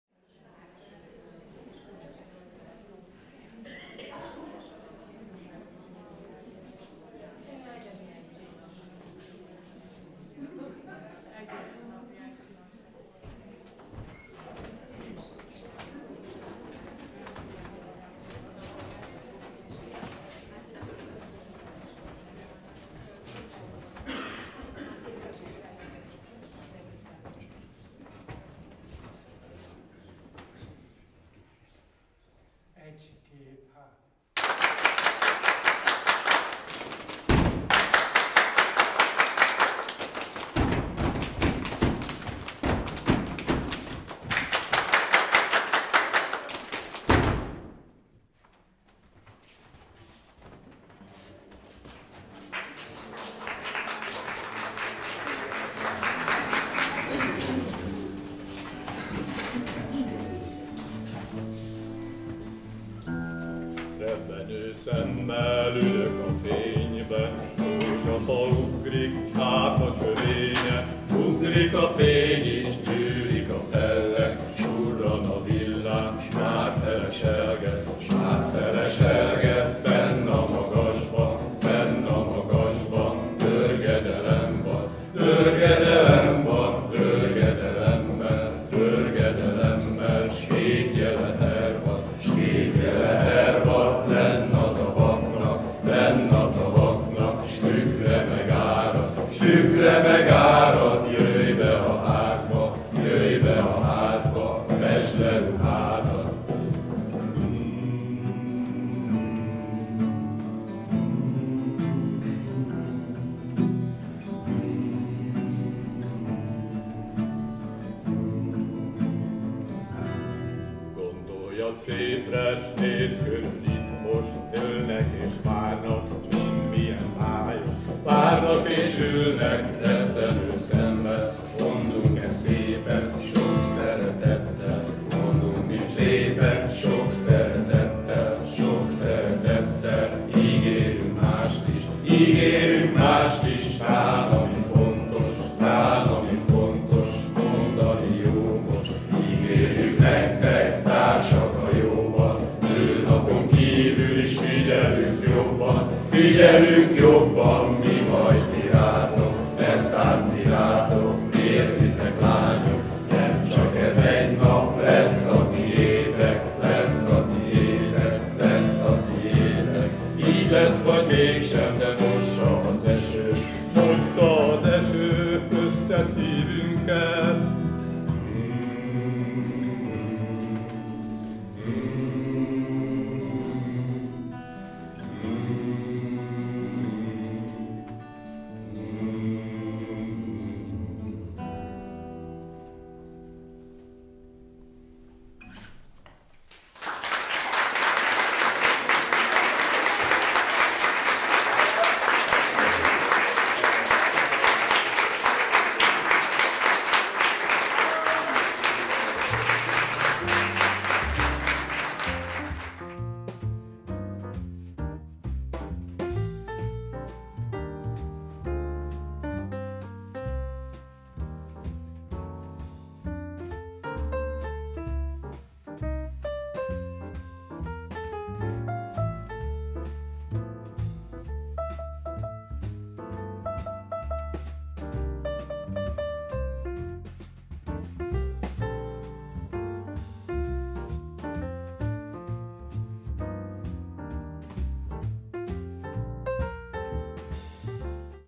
SZIG Nőnap 2020
A Soproni Széchenyi István Gimnázium tanári karának férfi tagjai immár többéves hagyományunkat követve ismét egy meglepetéssel kedveskedtek hölgy tanárainknak nőnap alkalmából.

Nézze újra a korábbi videókat!